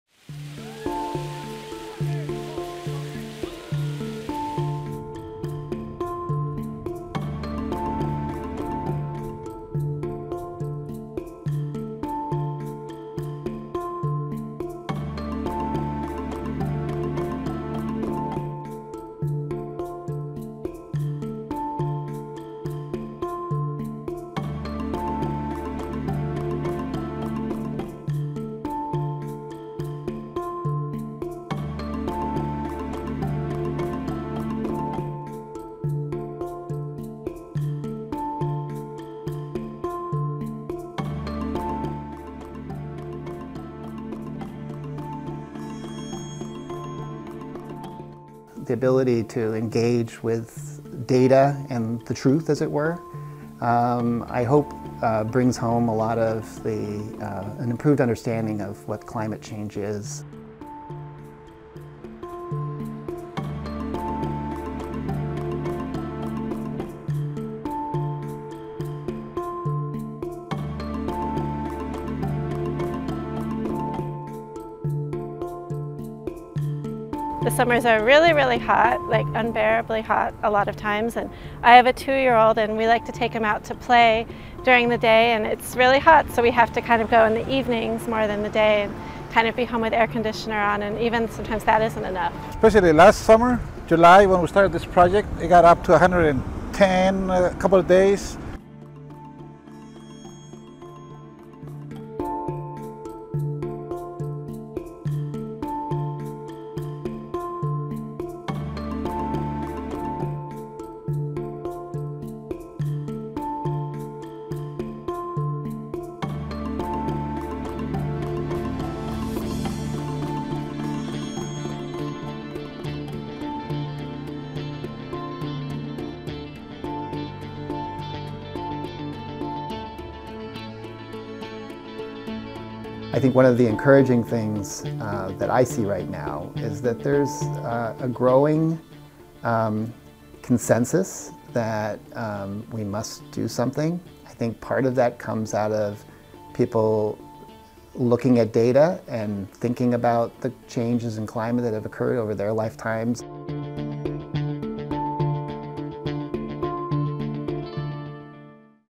AP finds 2:1 ratio of heat records to cold ones
A new Associated Press data analysis shows that over the past 20 years, Americans have been twice as likely to sweat through record-breaking heat rather than shiver through record-setting cold. (March 19) -- The Associated Press